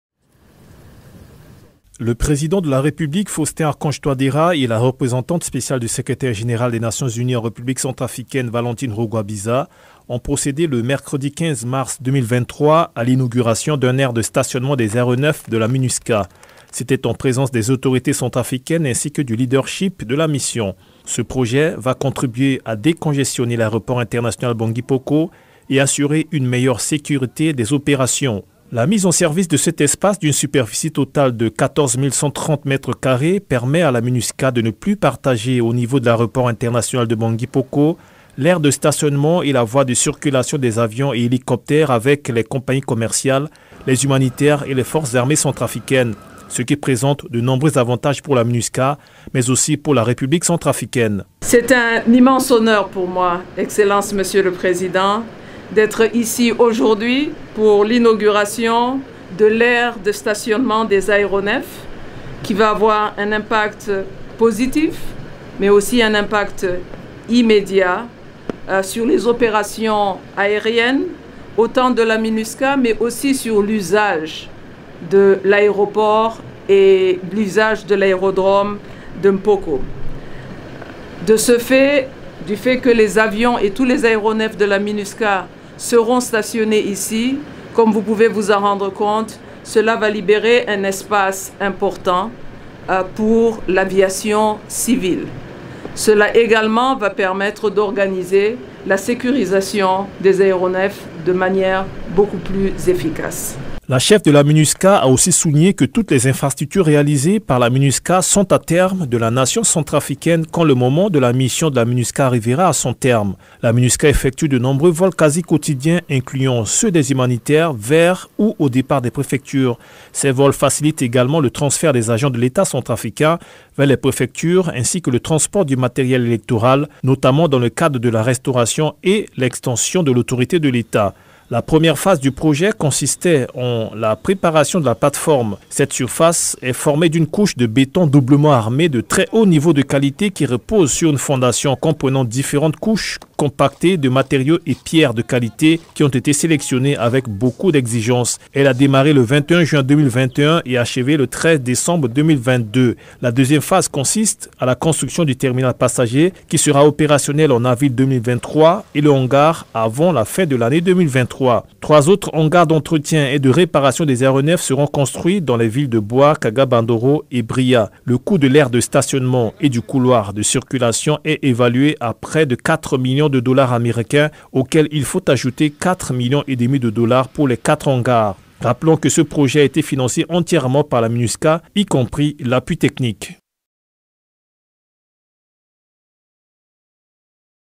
Une aire de stationnement des aéronefs de la MINUSCA à l’aéroport de Bangui M’poko
Le président de la République Faustin Archange Touadera et la Représentante spéciale du Secrétaire général des Nations Unies en République centrafricaine, Valentine Rugwabiza, ont procédé, le mercredi 15 mars 2023, à l’inauguration d’une aire de stationnement des aéronefs de la MINUSCA.
C’était en présence des Autorités centrafricaines ainsi que le leadership de la Mission. Ce projet va contribuer à décongestionner l’Aéroport international Bangui M’poko et assurer une meilleure sécurité des opérations. La mise en service de cet espace d’une superficie totale de 14.130 m2 permet à la MINUSCA de ne plus partager au niveau de l’aéroport international de Bangui M’poko l’aire de stationnement et la voie de circulation des avions et hélicoptères avec les compagnies commerciales, les humanitaires et les Forces armées centrafricaines ce qui présente de nombreux avantages pour la MINUSCA, mais aussi pour la République centrafricaine.